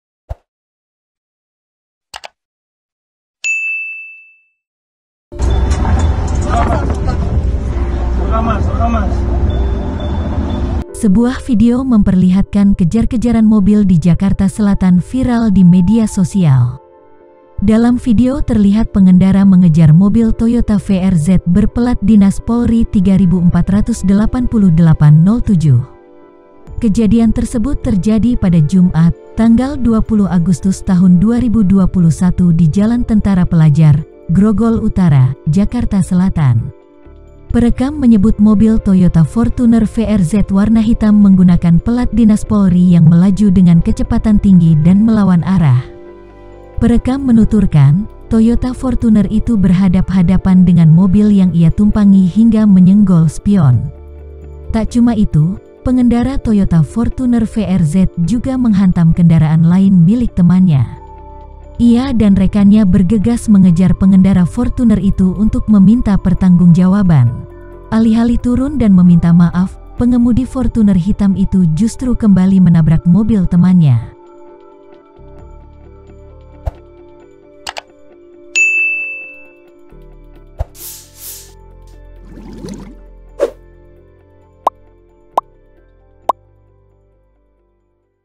Mobil Pelat Dinas Polri Dikejar Usai Tabrak Pengendara di Jakarta Selatan
Dalam video terlihat pengendara mengejar Mobil Toyota VRZ berpelat dinas Polri 3488-07.

Kejadian tersebut terjadi pada Jumat (20/8/2021) di Jalan Tentara Pelajar, Grogol Utara, Jakarta Selatan.

Perekam menyebut mobil Toyota Fortuner VRZ warna hitam menggunakan pelat dinas Polri yang melaju dengan kecepatan tinggi dan melawan arah.

Video terkini dan terbaru..